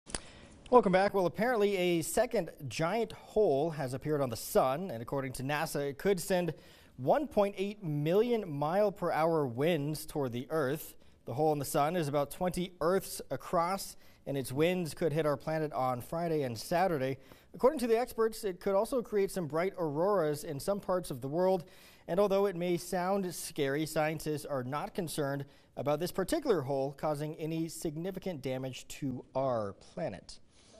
Second giant hole has appeared on the sun
Scientists are not concerned about this hole causing significant damage to our planet.